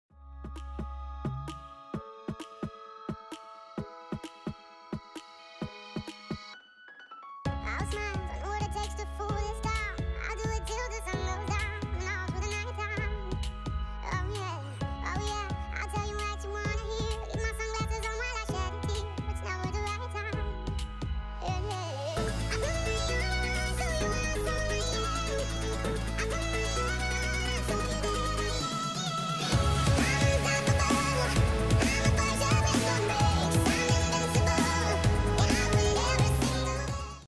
Lego Titanic Sinking ( Stop Motion 7. Tsunami Hits Titanic )
I've now decided I'm posting MONTHLY!! I'm thinking of different ideas which the Titanic could of done